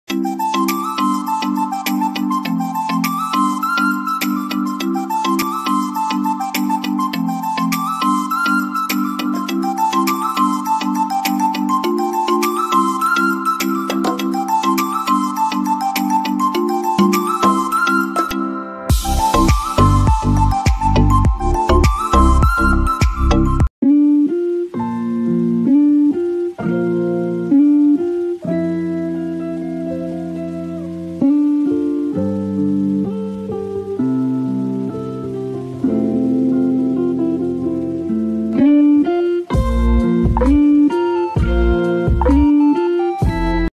硬币消失
原来魔术师是这样把硬币消失！！